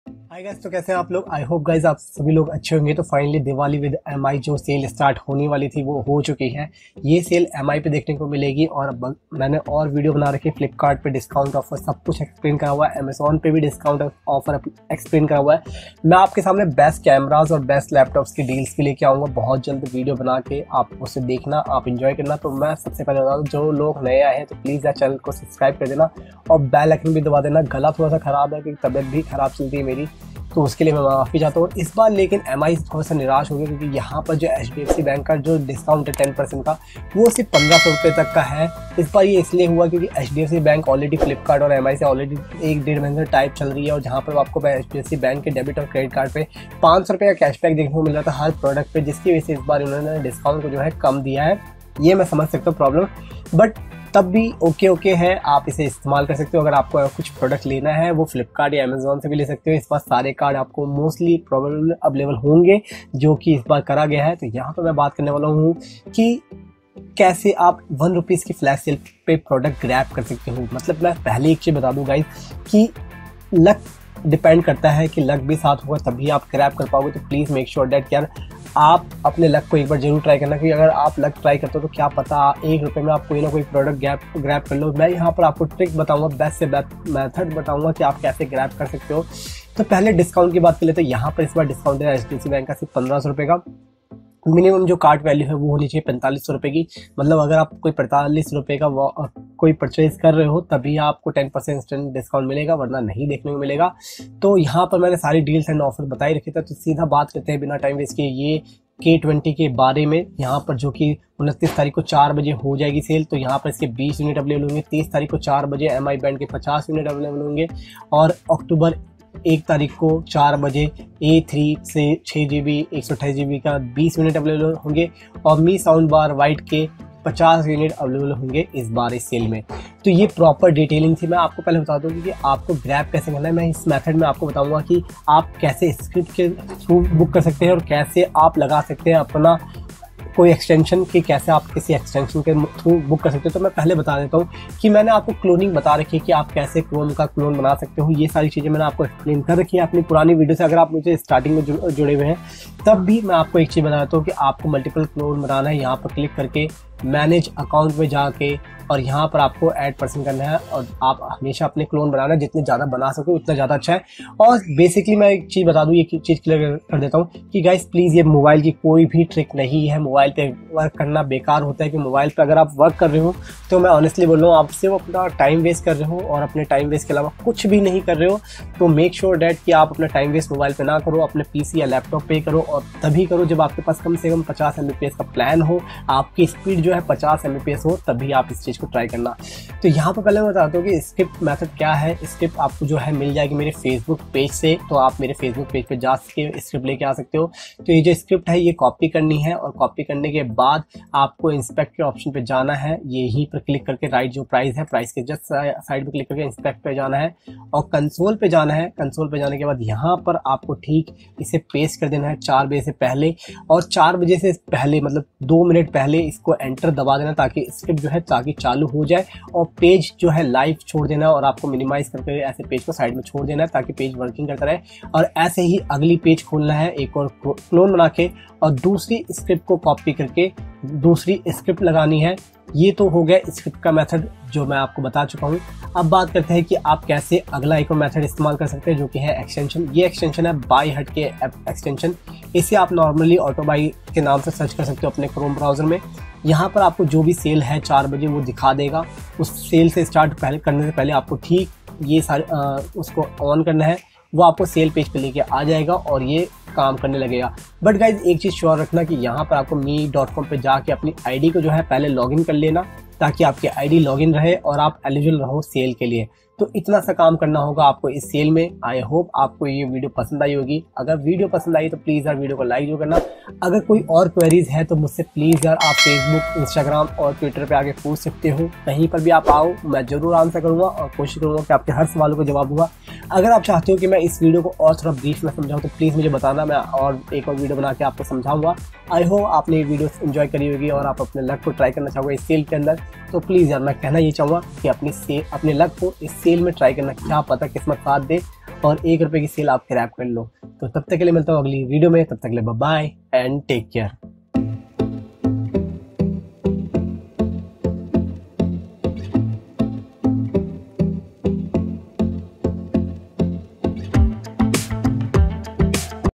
Mi 1 Rs Sale Script Trick 2022 - 20th September 2022
hi dosto to aaj ki iss video me hum baat karenge mi ki diwali sale ke baare me jaha aapko dekhne ko milega 29th September se 4th october tak 1 rs sale. Isiliye iss video me hum baat karenge iss sale me kese 1 rs me redmi k20 kese grab kar sakte hai aur baaki product kese grab kar paaoge.

Keyword Use In this video -